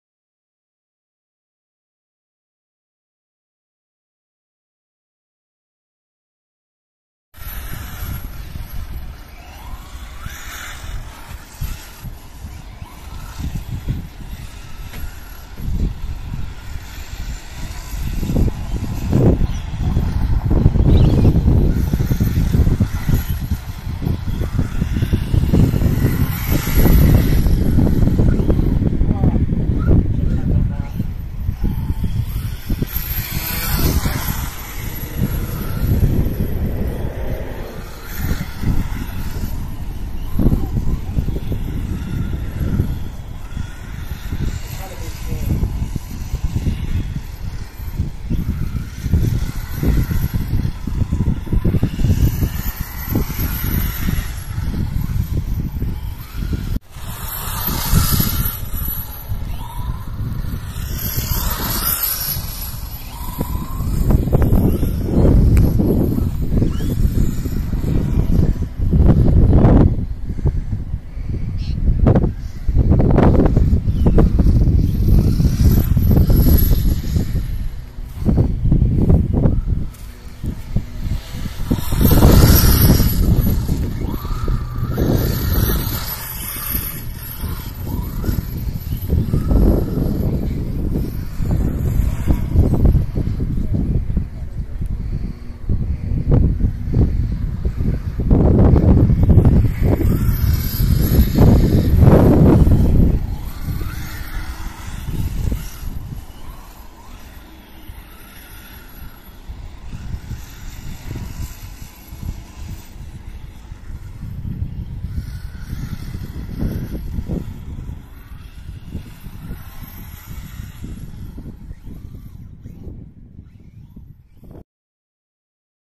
H.K Rattler Track run 🐍
Hobby King Rattler 3S track run with Kforce semi slick road tyres.
No centre diff.